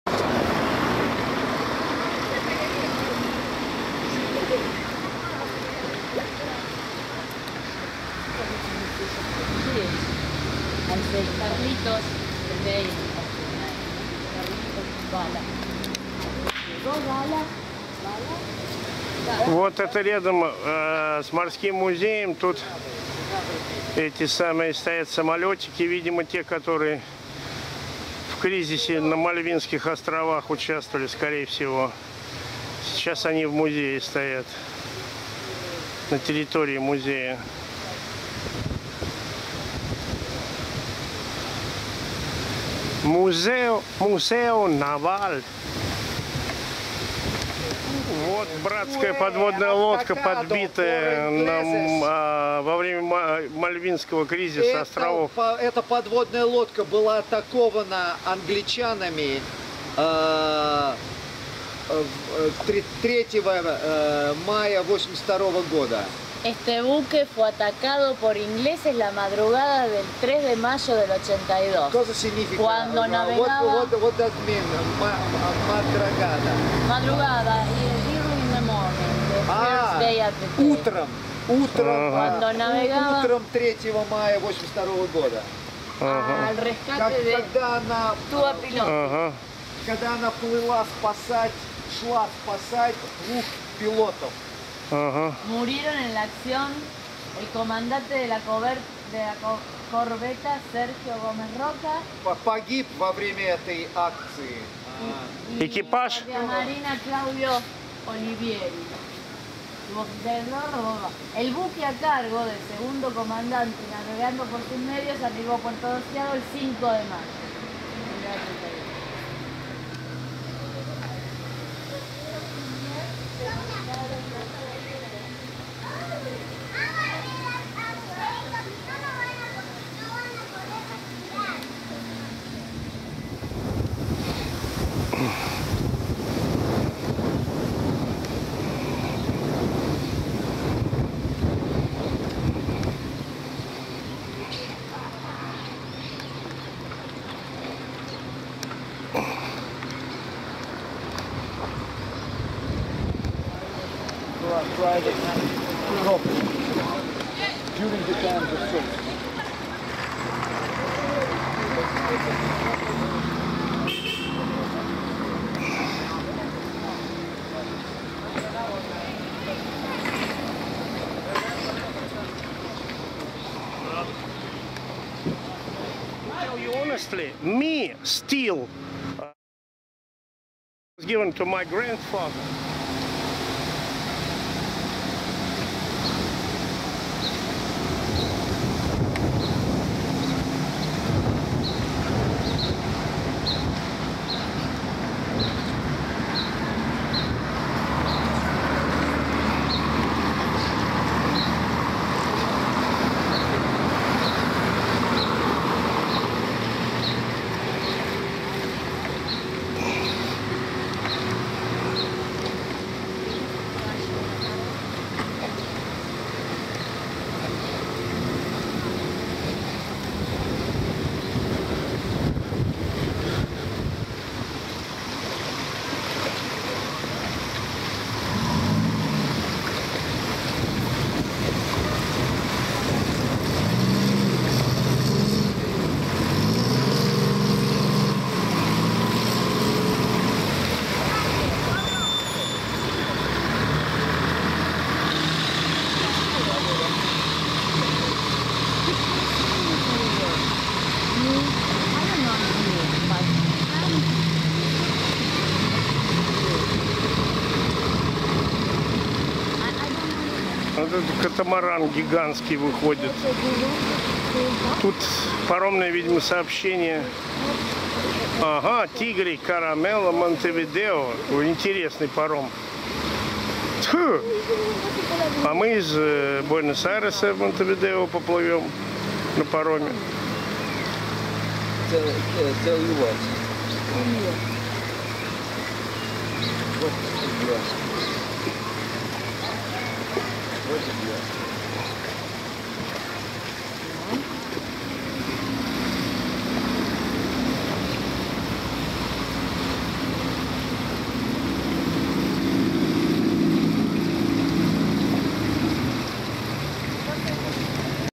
ТИГРЕ / TIGRE (2)
Курортный городок Тигре, в 30 км на Северо-Запад от Буэнос-Айреса.